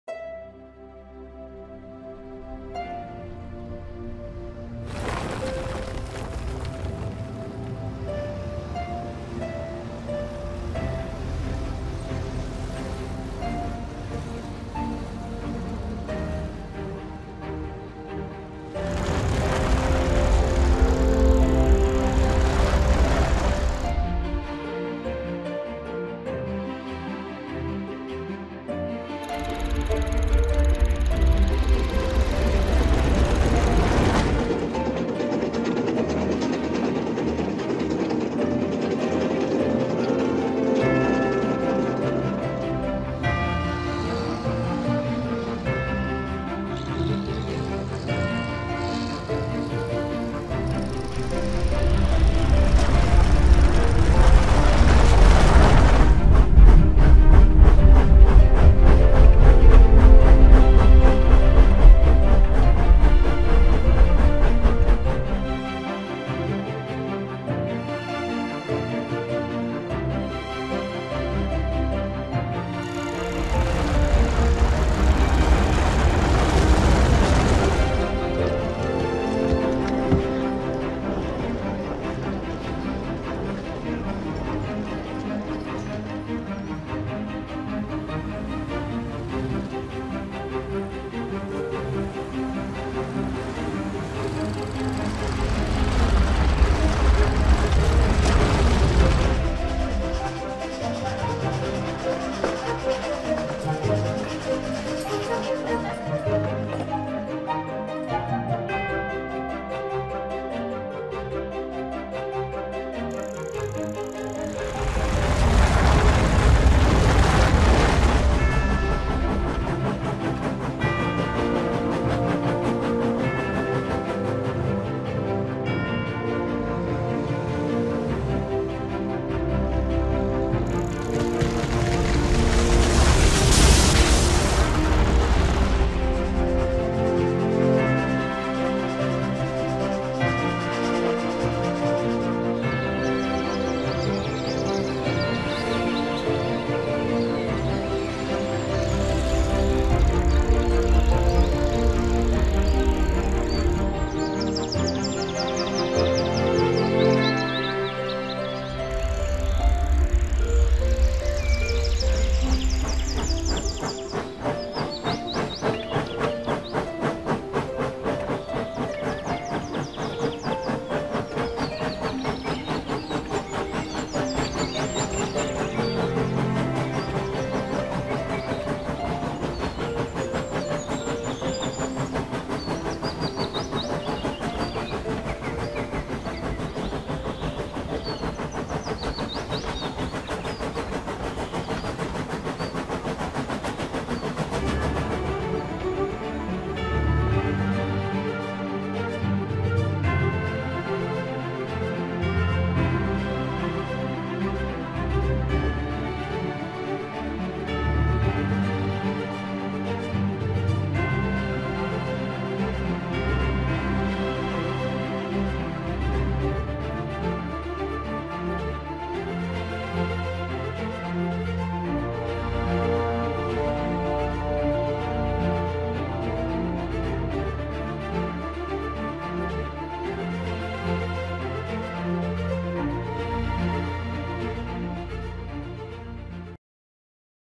Lost Stations: Herefordshire
Views of Stations lost through time in the county of Herefordshire! Credit to the image owners featured.